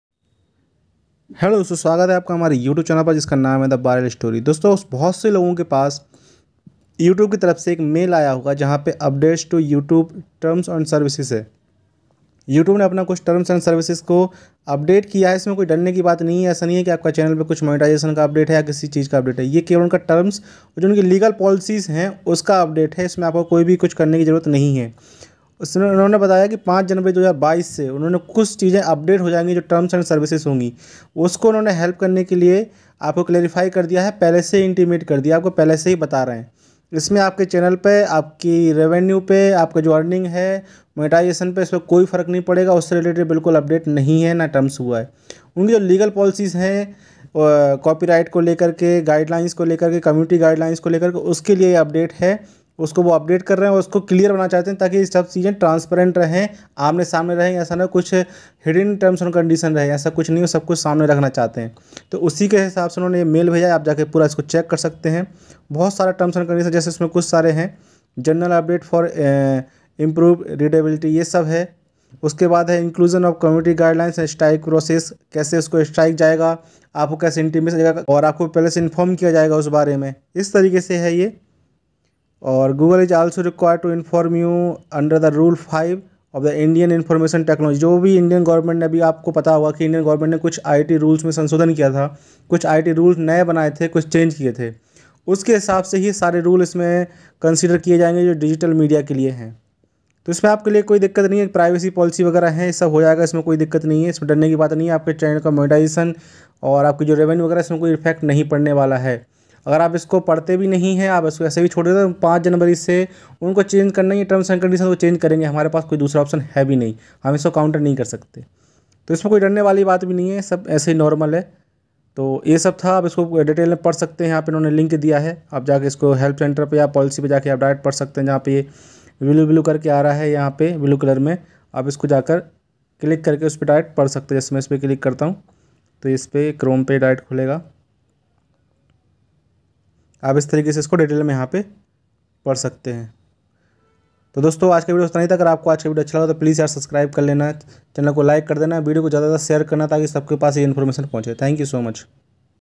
Updates to YouTube Terms of Service - Mail from YouTube in Hindi details - The Viral Story
Updates to YouTube's Terms of Service
- Mail from YouTube in Hindi details

On January 5, 2022, we're updating our Terms of Service ("Terms") to help clarify and make it easier to understand what to expect as you use the service. These changes won't affect the way you use YouTube. You can review a summary of the changes and the

At a glance, this update covers:

More clarity on what to expect: We're providing more transparency about how we develop, improve, and update our service, including more detail about the reasons we make changes and updates, and the advanced notice we provide to you.
General updates for improved readability: While our Terms remain a legal document, we've done our best to make them easier to understand. This includes reorganizing some sections (such as Account Suspension and Termination) and rewording others (such as Changing this Agreement).
Inclusion of the Community Guidelines strikes process: Your use of YouTube has always been subject to the Community Guidelines and its strikes process, but we're now explicitly

On January 5, 2022, we're updating our Terms of Service ("Terms") to help clarify and make it easier to understand what to expect as you use the service. These changes won't affect the way you use YouTube. You can review a summary of the changes and the updated Terms here.

Inclusion of the Community Guidelines strikes process: Your use of YouTube has always been subject to the Community Guidelines and its strikes process, but we're now explicitly including the Community Guidelines strikes information in the Terms to increase transparency. There are no changes to how Community Guidelines strikes operate, or when a channel or content might receive a strike.

Google is also required to inform you under Rule 5 of the Indian Information Technology (Intermediary Guidelines and Digital Media Ethics Code Rules, 2021) Rules, 2021 that if you are a publisher of news or current affairs content, you are required to furnish the details of your accounts on YouTube to the Ministry of Information and Broadcasting, Government of India.

Please make sure you read these updates and remember that the Terms and Google's Privacy Policy apply to your use of YouTube. Violation of these agreements may result in warnings or further action including loss of access or termination of all or part of your account.

By continuing to use YouTube after this date, you are agreeing to the updated Terms. Note, if you allow your child to use YouTube Kids, or manage your child's use of YouTube through Family Link, take a moment to talk to them about these changes.

Thank you for being part of our global communications

You have received this email to update you about important changes to YouTube's Terms of Service.